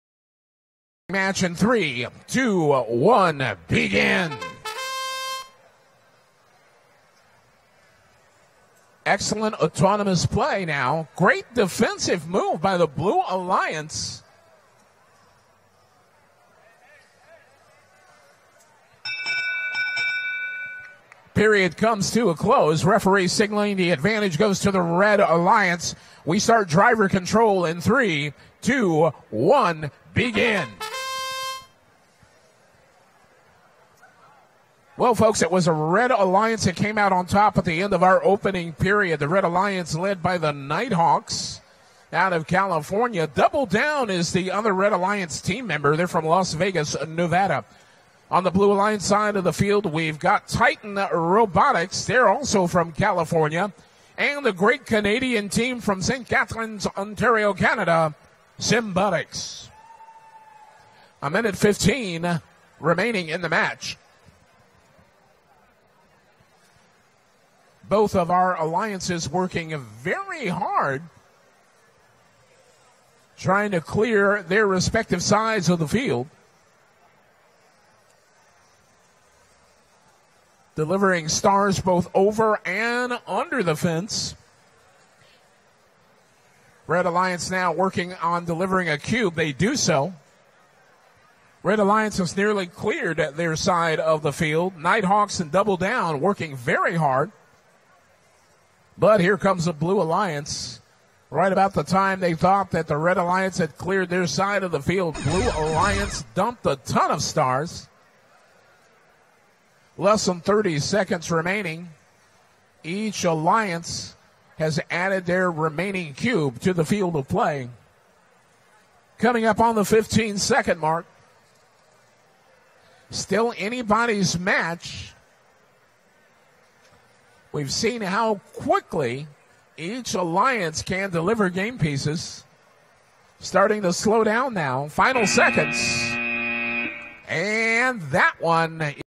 2017 VRC Engr Q60 - 569D 64C vs 563 1114Y - 18 to 26
VEX Worlds 2017 - VRC High School - Engineering Division - Qualifiers Match 60

Red - Score 18:
569D (NightHawk Robotics Club, Murrieta Valley High School, Murrieta, California, United States)

64C (Double Down, Ed W. Clark High School, Las Vegas, Nevada, United States)

Blue - Score 26:
563 (Titan Robotics, Nipomo High School, Nipomo, California, United States)
||  Engineering Rank:35 . Wins:6 . Loses:4 . WP:12 . AP:16 . SP:144

1114Y (Simbotics, Governor Simcoe Secondary School, St Catharines, Ontario, Canada)